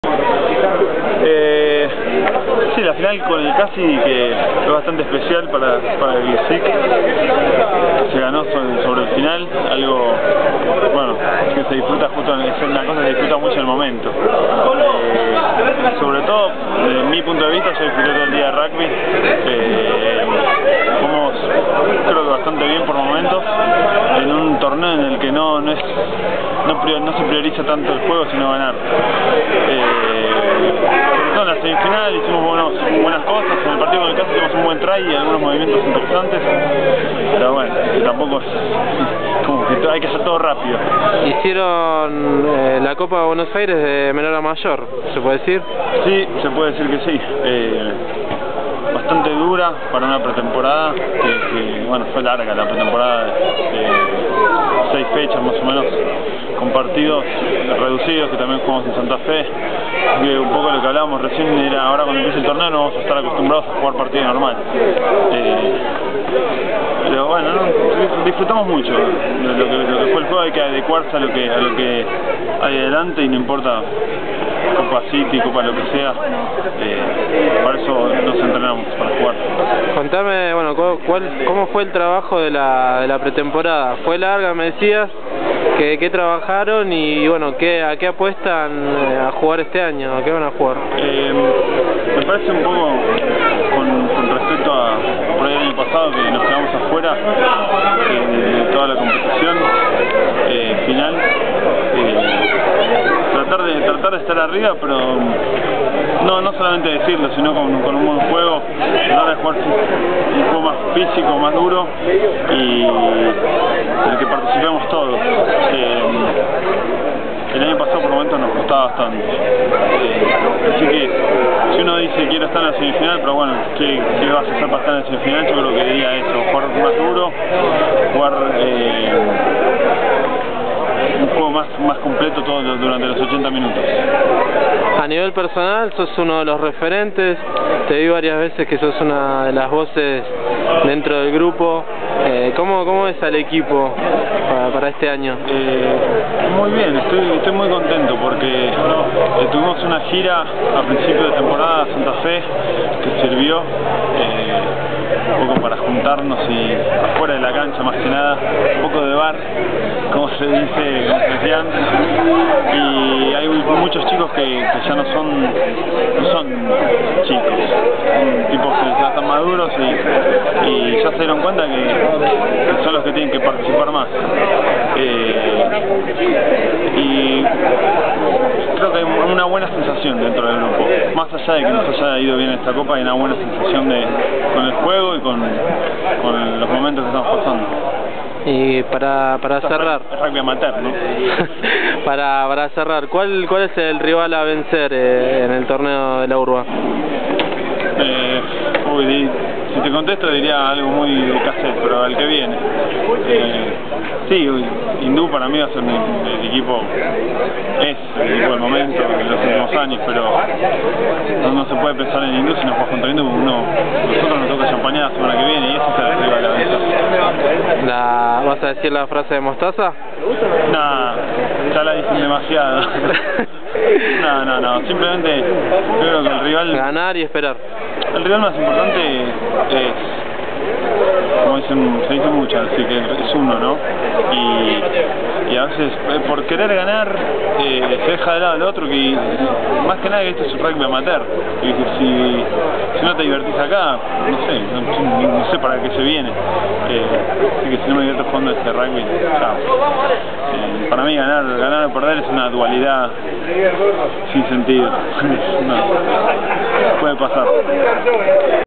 Tomás De Vedia en Rugby Show!
Luego de quedarse con la Copa Buenos Aires con su equipo, el jugador de SIC analizó la actualidad del Zanjero y nos contó sus expectativas para esta temporada